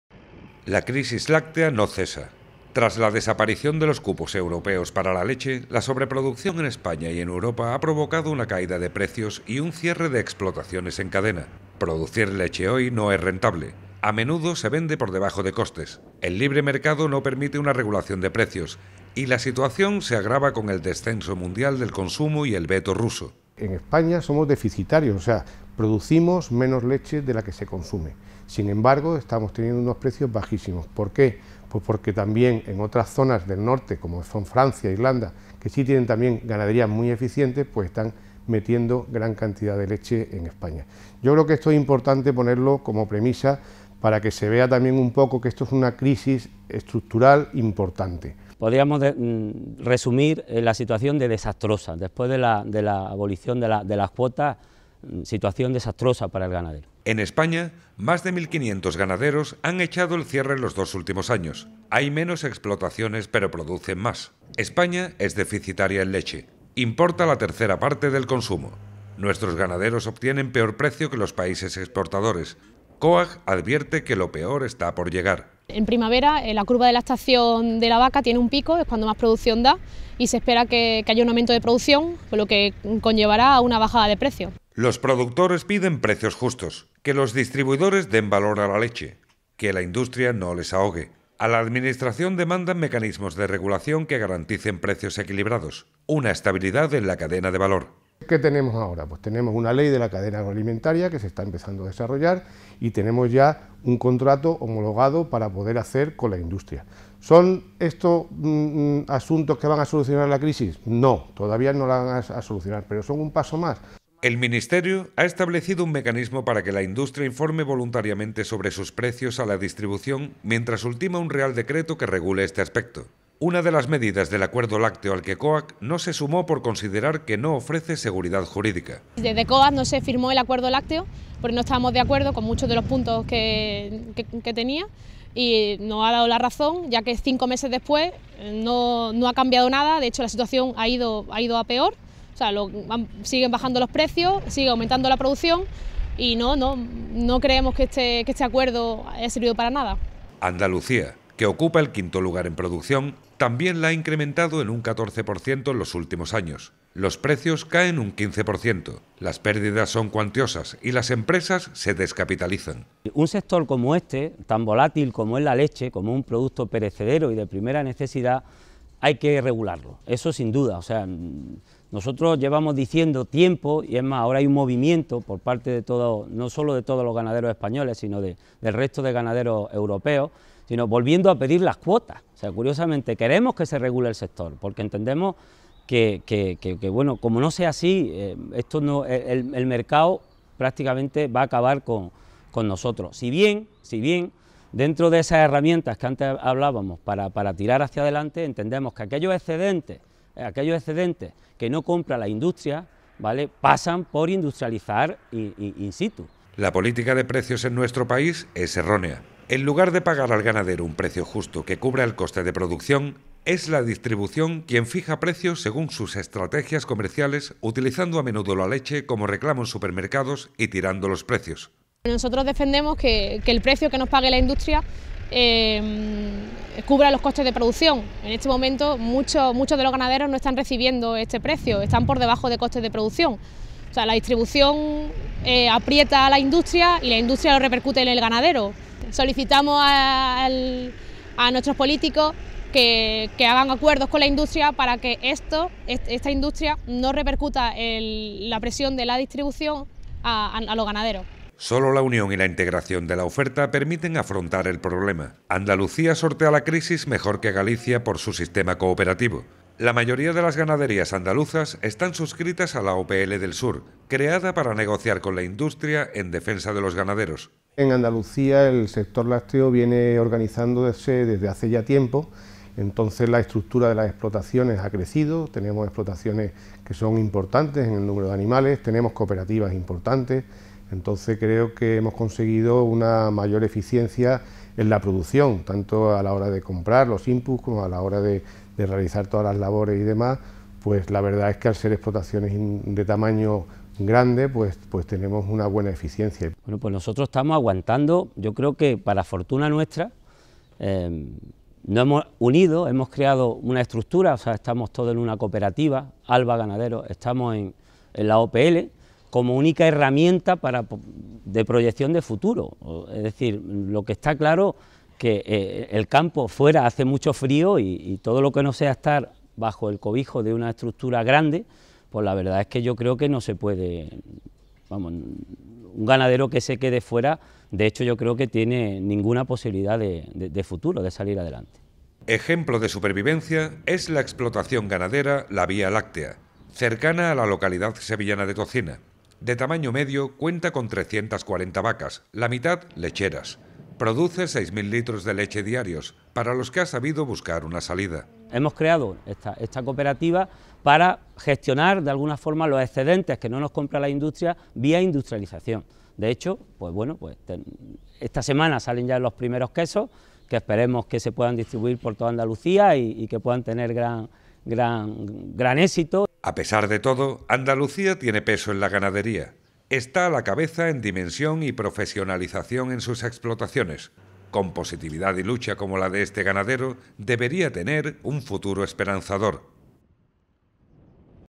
Crisis láctea en Andalucía
Los ganaderos andaluces también sufren los efectos de la crisis láctea que afecta a toda España. Una situación desastrosa que se produce tras la retirada de las cuotas de leche en Europa. La consecuencia, una sobreproducción de leche, una fuerte caída de precios y el cierre de muchas explotaciones.